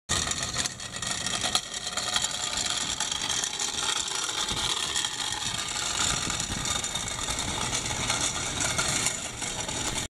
Test Ride of the Second Realized Optimal Lateral Handling Bicycle
Slight downhill coast with front caster in place.